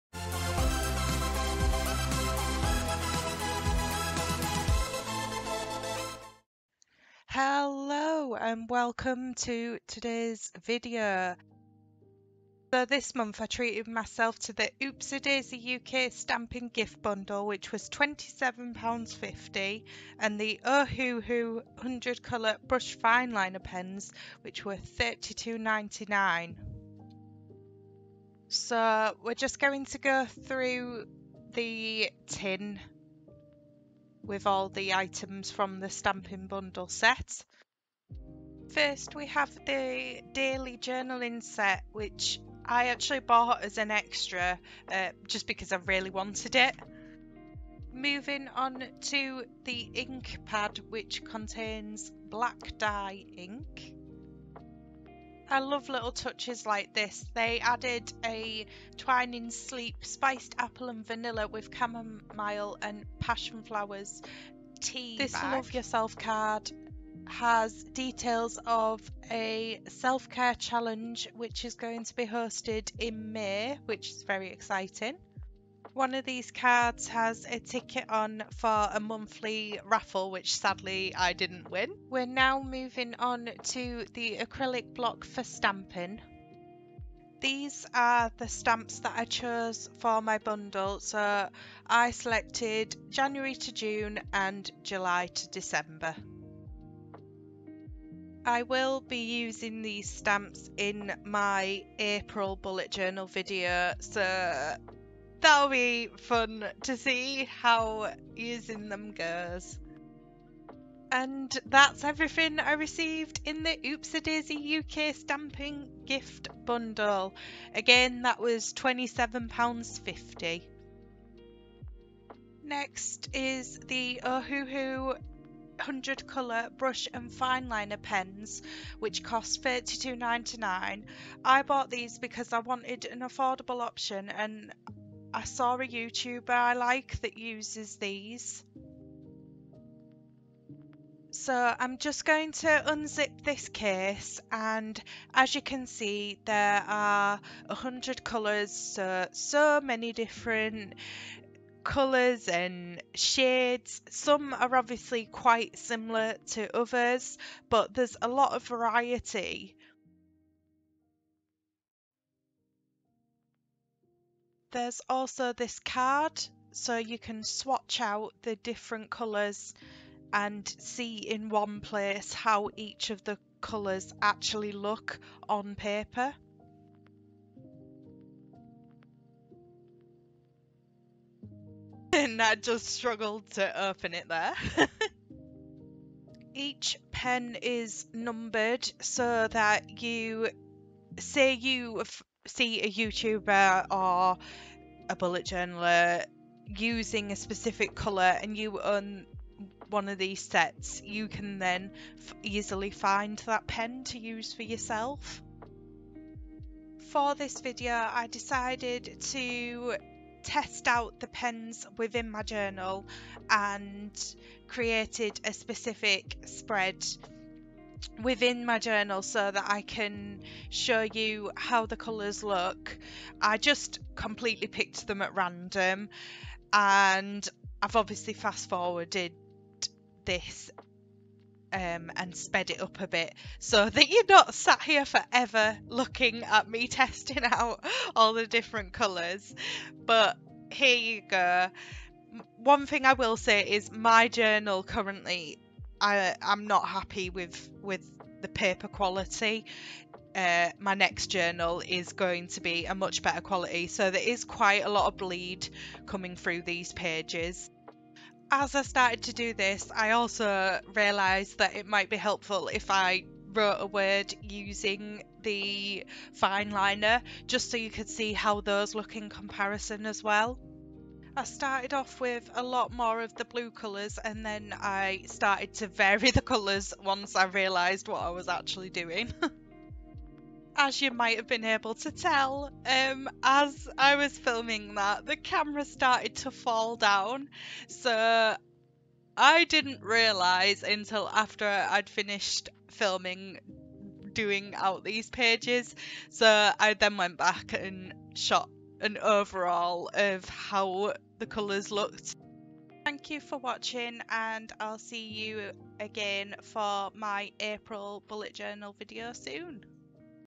Bullet Journal - Oops A Daisy UK Stamping Gift Bundle & Ohuhu 100 Colour Brush/Fine liner Pens
I recently treated myself to the Oops A Daisy UK Stamping Gift Bundle and the Ohuhu 100 Colour Brush & Fine liner Pens! I thought I'd put together this video showing you what I received in each. I also test out some of the Ohuhu pens towards the end of the video!